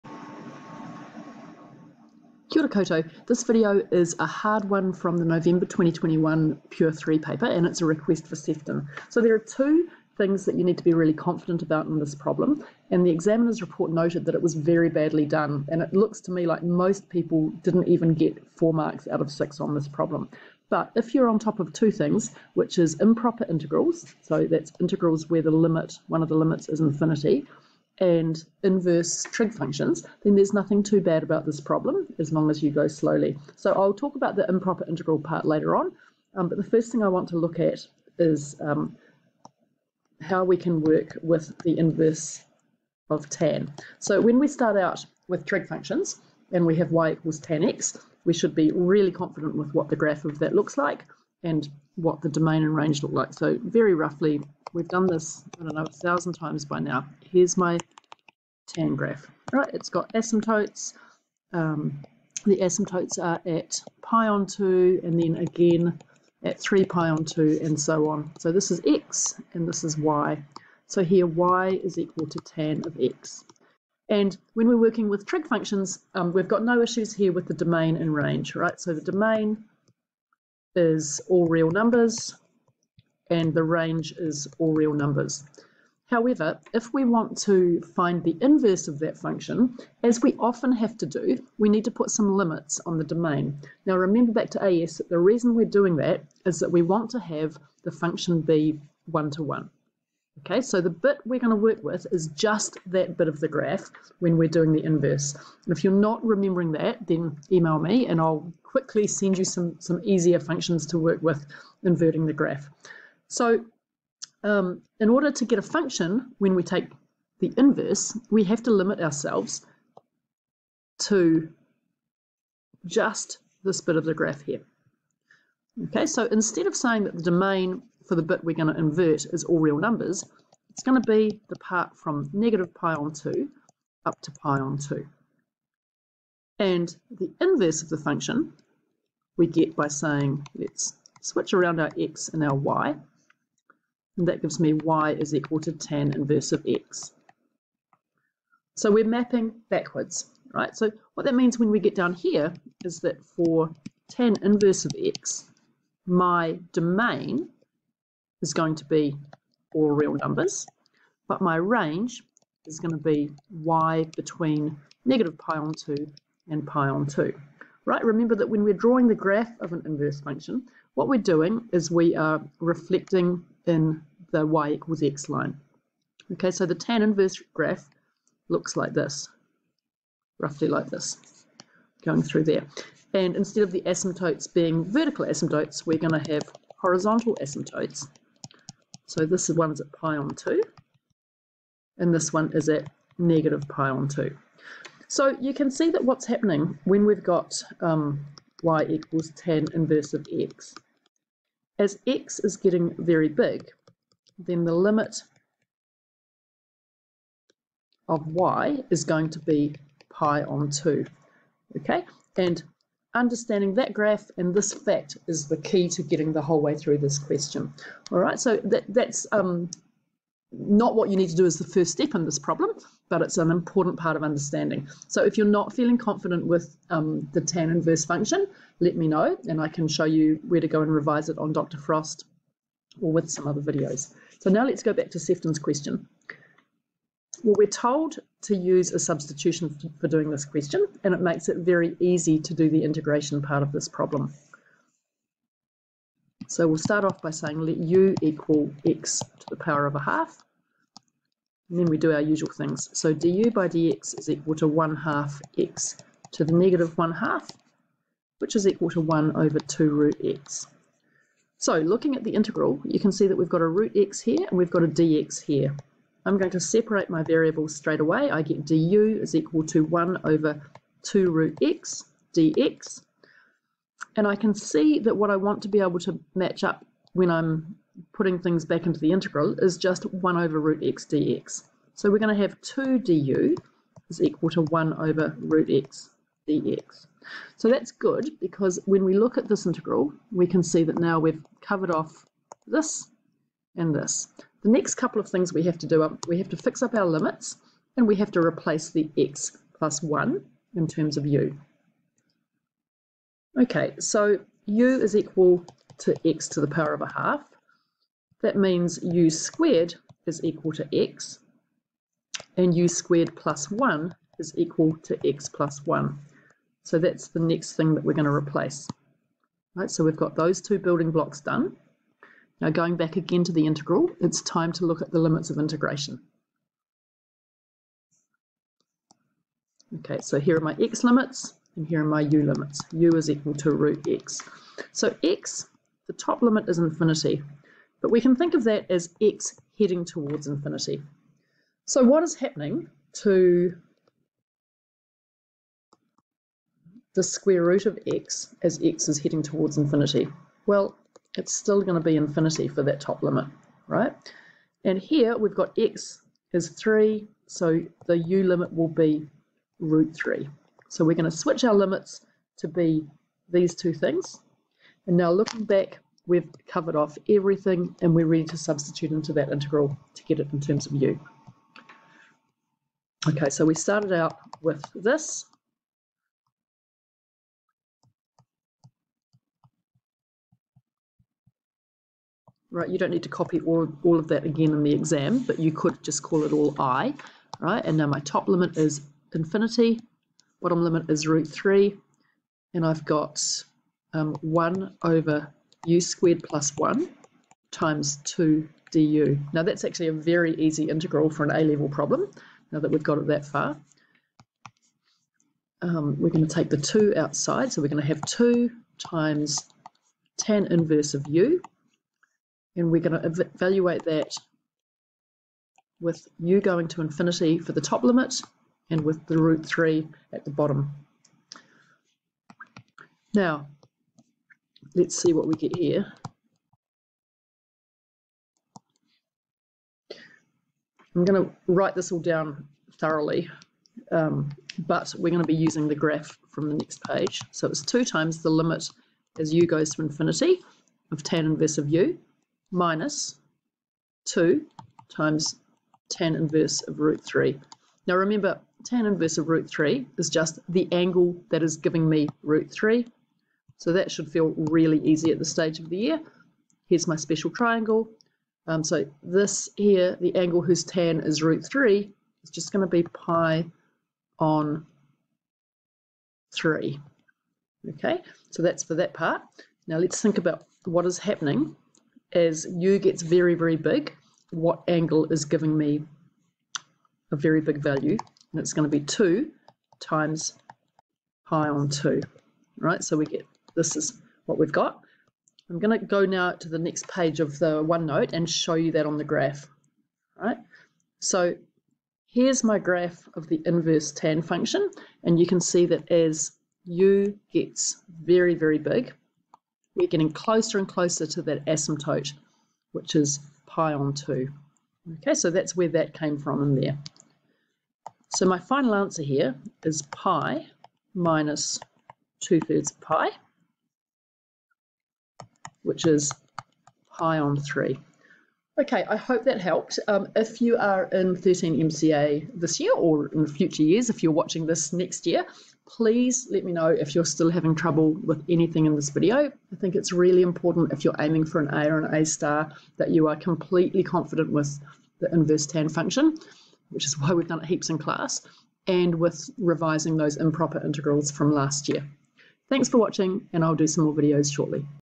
13MCA - Pure 3 Nov 2021 Q4 - Integration - hard problem - inverse tan and improper integral
Nice substitution problem needing understanding of inverse tan function and improper integrals. Noted as badly done in the Examiner's Report!